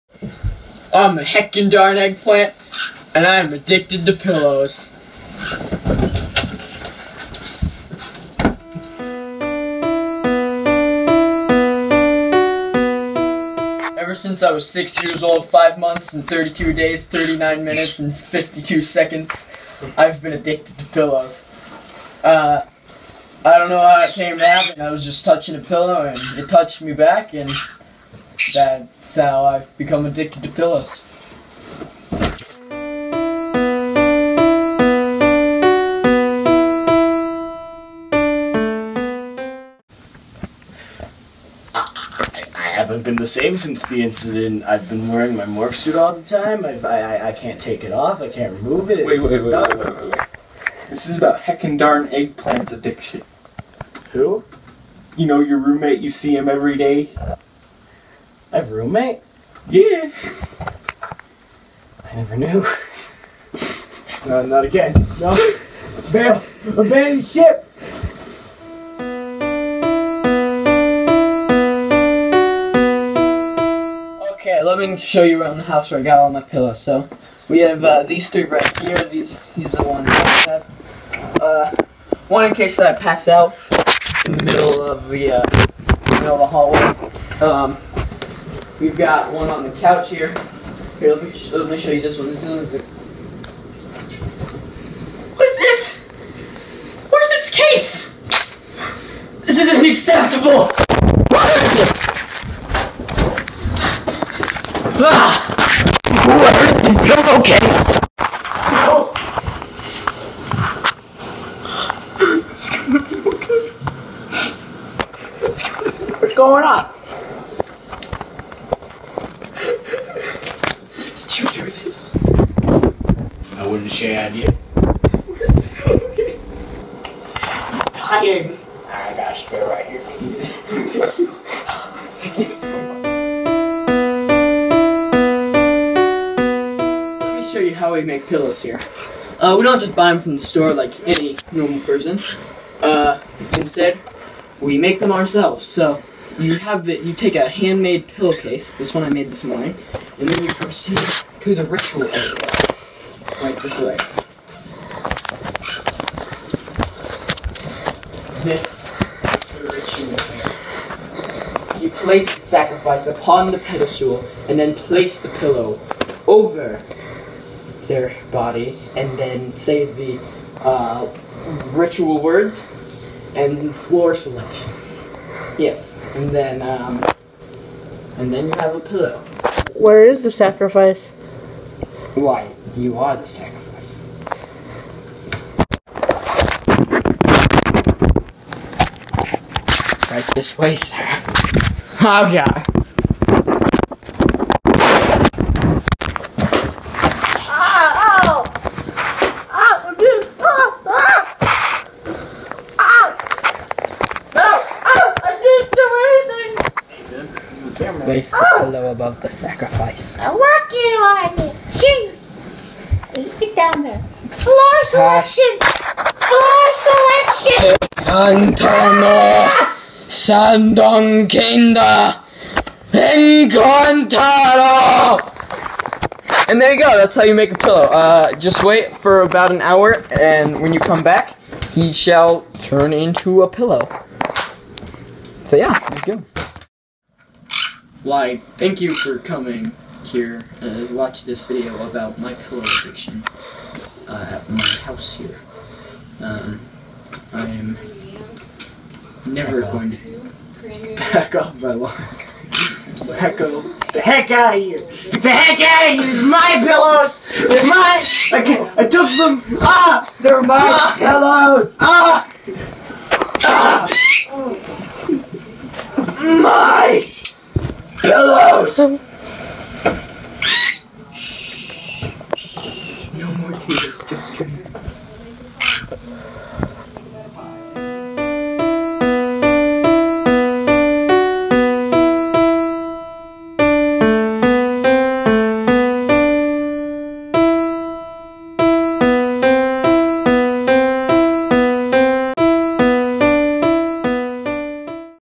My Pillow Addiction | My Strange Addiction Parody (by a Heckin' DarnEggplant)
People have very strange addictions, but this has gone TOO far.